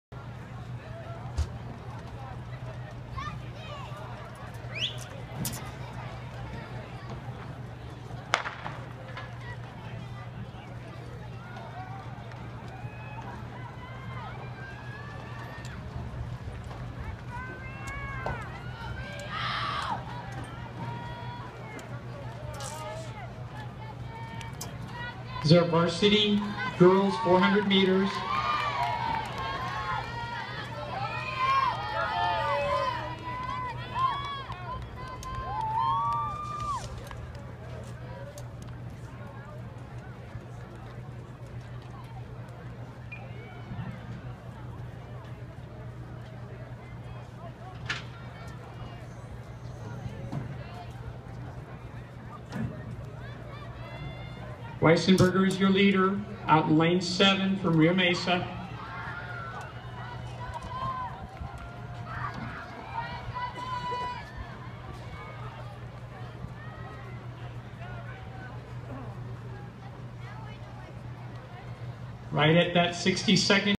RMHS vs Oak Park - Girls 400m
Dev runs 61.56, 1st overall